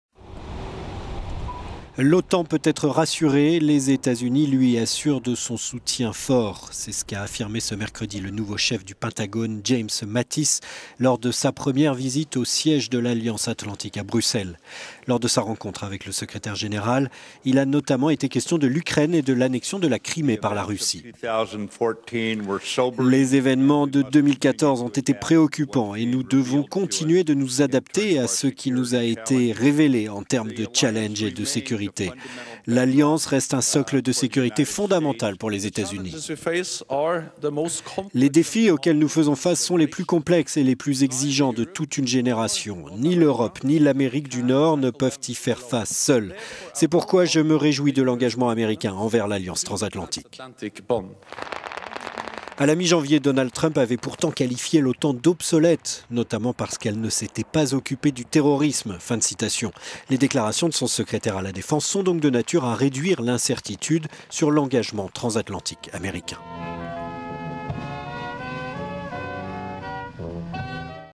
Le Pentagone rassure l'OTAN et maintient son engagement transatlantique
L'OTAN peut être rassuré, les Etats-Unis lui assurent de son "soutien fort". C'est ce qu'a affirmé ce mercredi le nouveau chef du Pentagone James Mattis lors de sa première visite au siège de l'Alliance atlantique à Bruxelles.

Lors de sa rencontre avec le Secrétaire général, il a notamment été question de l'Ukraine et de l'annexion de la Crimée par la Russie :

James Mattis, Secrétaire  américain à la Défense :
"Les évènements de 2014 ont été préoccupants et nous devons continuer de nous adapte…